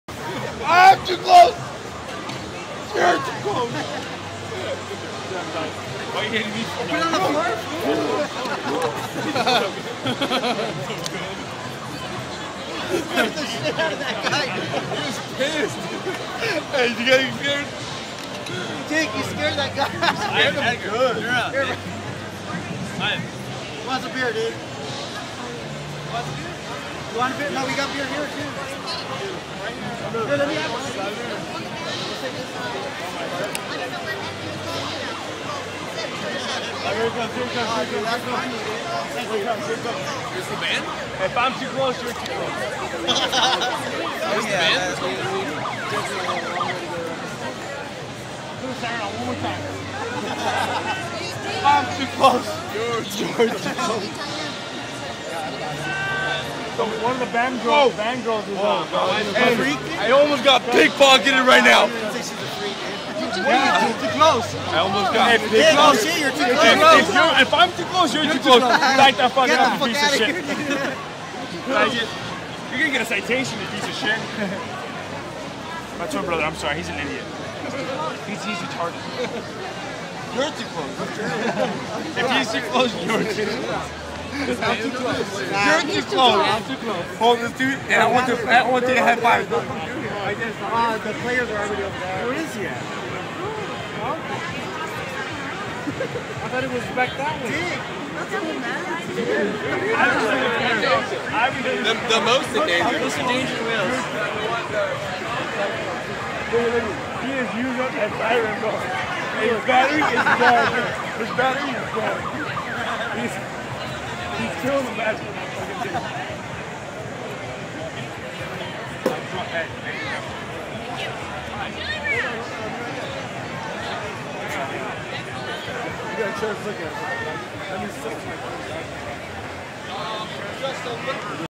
"If I'm too close, you're too close"
Some USC security Nazi dude on a Segway kept driving up to people and saying, "If I'm too close, you're too close." Of course, Ivan and Edgar took that phrase and ran with it -- hilarity ensued.